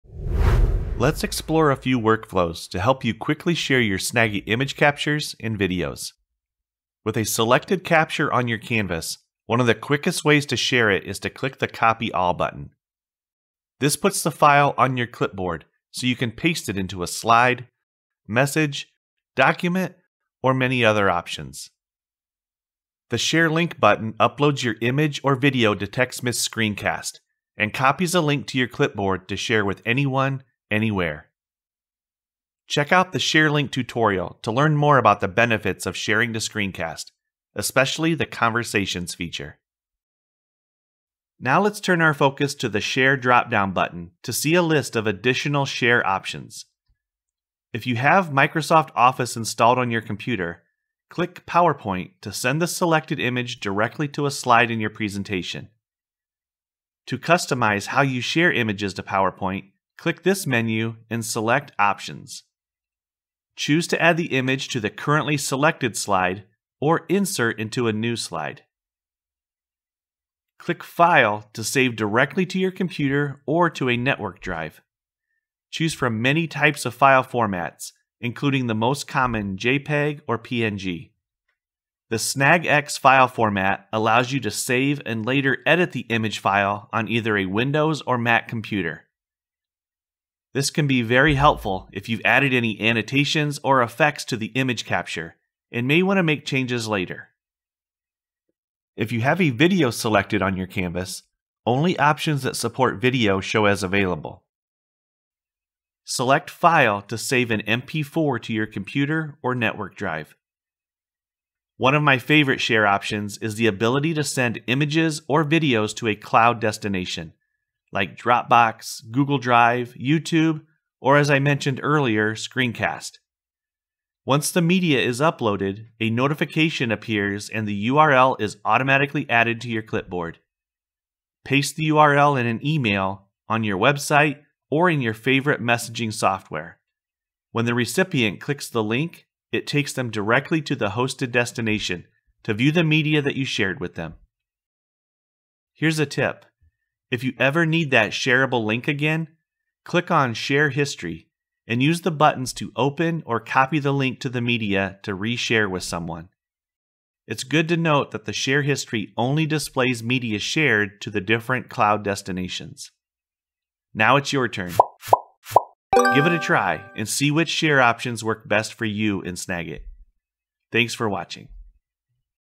Share Your Captures in Snagit
Sharing your media means getting it where you need it, when you need it. The Share menu contains a number of places for you to quickly send your media.